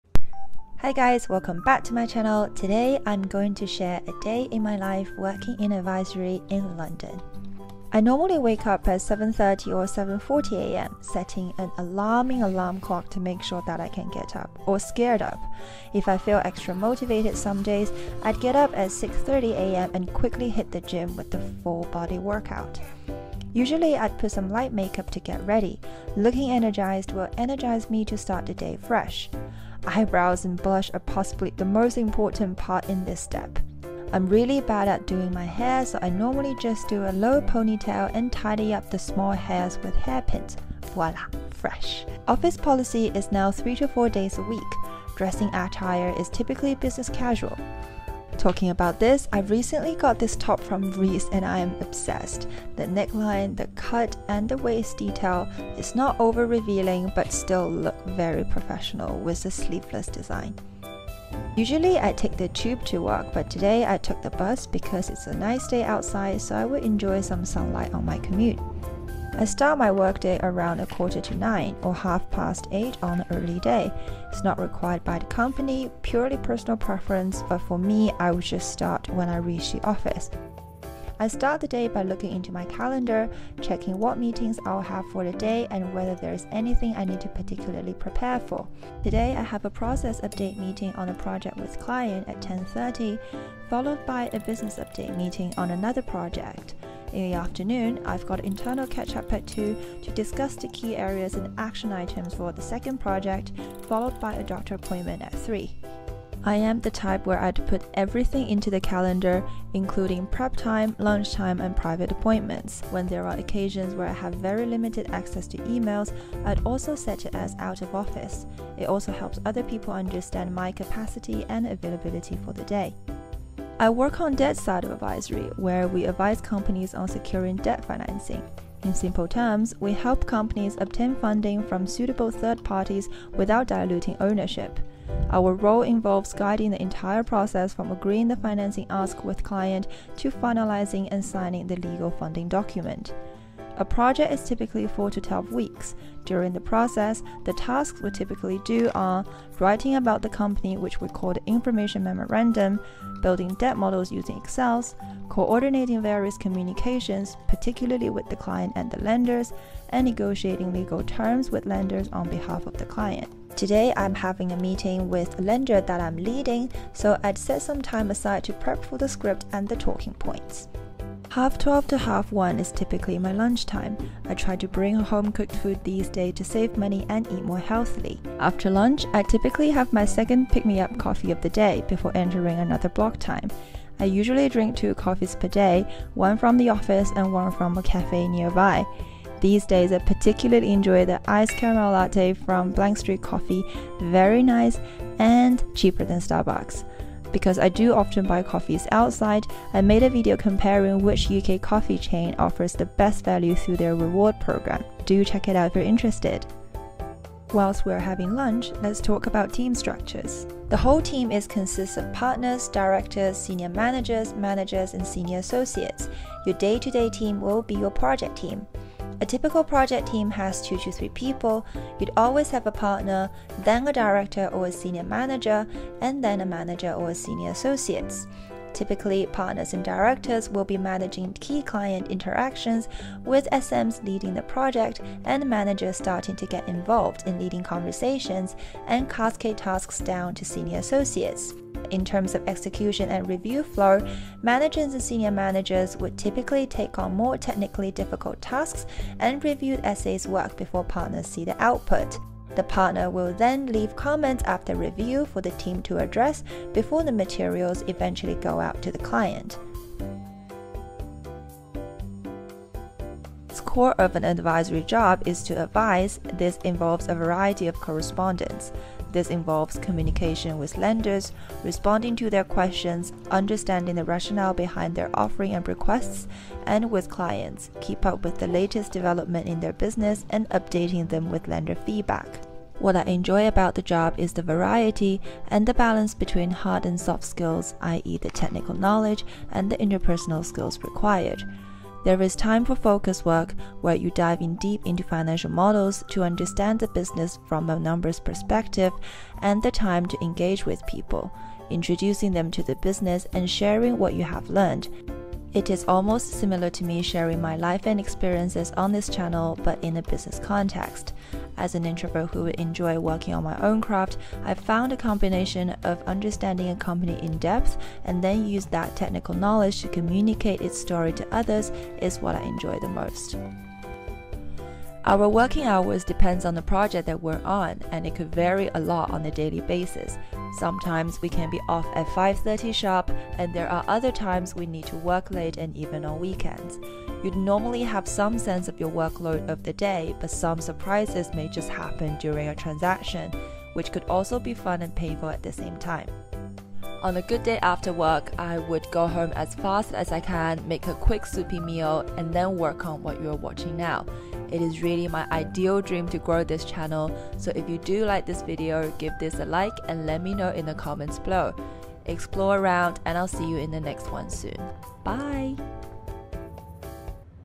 A day in my life working as a debt advisor in London
Today I am sharing a day in my life working at a boutique advisory firm in London. In this video I talk about a typical day in my life, including team structures, work flows, key skills required, and what I enjoy about my job. Hope you find it helpful!